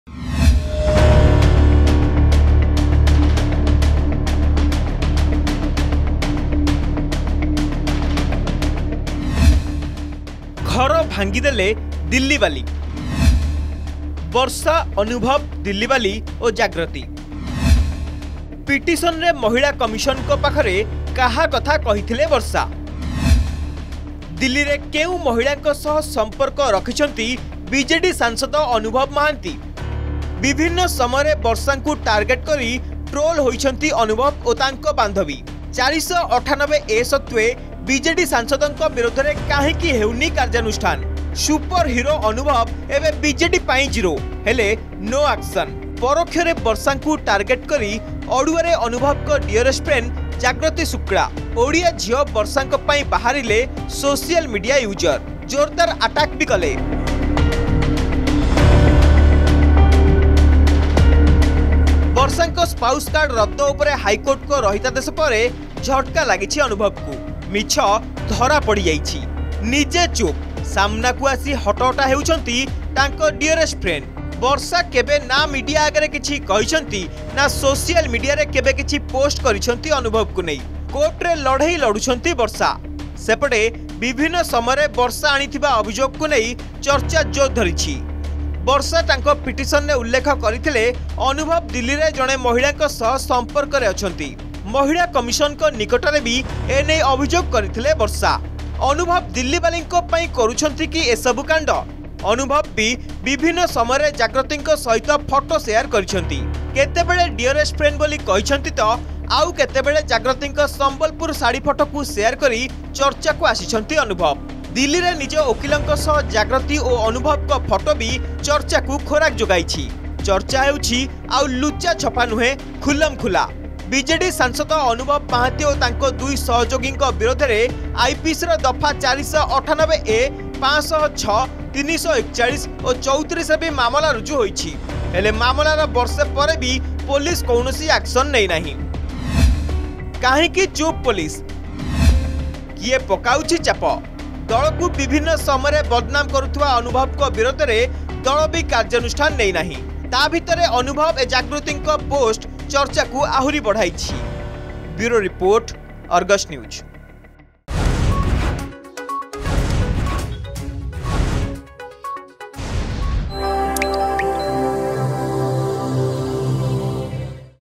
Anubhav Mohanty's dearest friend Jagrati Shukla got trolled for Targeting Barsha Priyadarshini
MP Anubhav Mohanty's dearest friend, Jagrati Shukla, got trolled on social media for targeting Barsha Priyadarshini on spouse card. Social media users came out in support of Ollywood actress Barsha Priyadarshini.